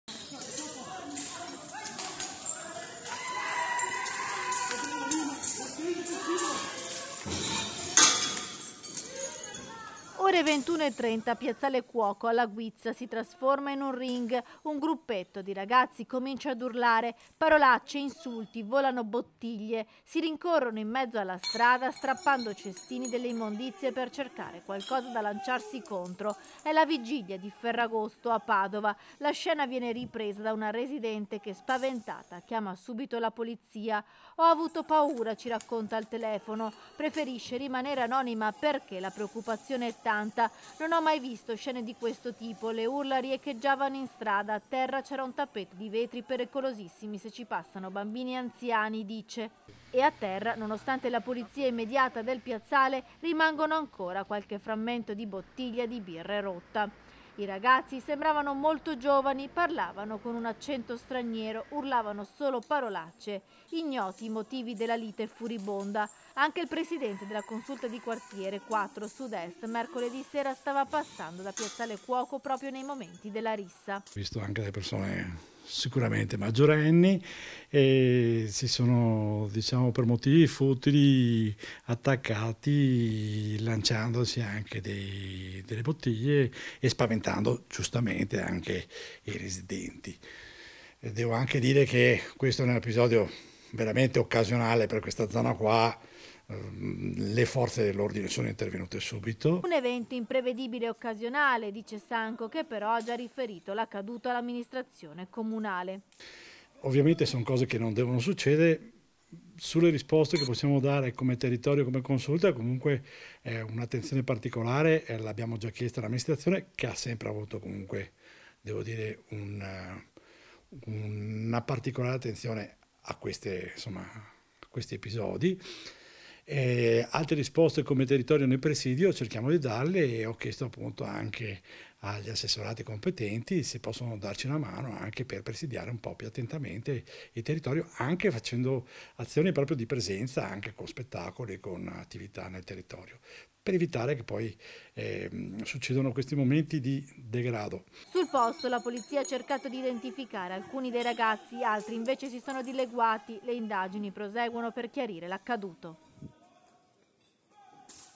RISSA A BOTTIGLIATE TRA RAGAZZI ALLA GUIZZA: E' CACCIA AI RESPONSABILI | 17/08/2024
A3 NEWS Veneto 17/08/2024 - PADOVA - La cronaca ci porta a parlare ancora di risse tra giovanissimi a Padova. Lo scontro questa volta nel quartiere Guizza la sera della vigilia di Ferragosto.   || Ore 21.30 Piazzale Cuoco alla Guizza si trasforma in un ring, un gruppetto di ragazzi comincia ad urlare, parolacce, insulti volano bottiglie, si rincorrono in mezzo alla strada strappando cestini delle immondizie per cercare qualcosa da lanciarsi contro. E' la vigilia di Ferragosto a Padova. La scena viene ripresa da una residente che spaventata chiama subito la polizia. "Ho avuto paura" ci racconta al telefono, preferisce restare anomina perchè la preoccupazione è tanta "non ho mai visto scene di questo tipo, le urla riecchieggiavano in strada, a terra c'era un tappetto di vetri pericolosissimi se ci passano bambini e anziani" dice. E a terra nonostante la pulizia immediata del piazzale, rimangono ancora qualche frammento di bottiglie di birra rotte. I ragazzi sembravano molto giovani, parlavano con un accento straniero, urlavano solo parolacce. Ignoti i motivi della lite furibonda. Anche il presidente della Consulta di Quartiere 4 Sud Est mercoledì sera stava passando da piazzale Cuoco proprio nei momenti della rissa. Un evento impredibile e occasionale, dice Sanco che però ha già riferito l'accaduto all'amministrazione comunale.Sul posto la polizia ha cercato di idenficare alcuni dei ragazzi, altri invece si sono dileguati. Le indagini proseguono per chiarire l'accaduto. - Intervistati ALESSANDRO STANCO (Presidente Consulta Quartiere 4 Sud-Est) (Servizio di Valentina Visentin) - Segui Antenna Tre anche sul digitale terrestre! Visita il sito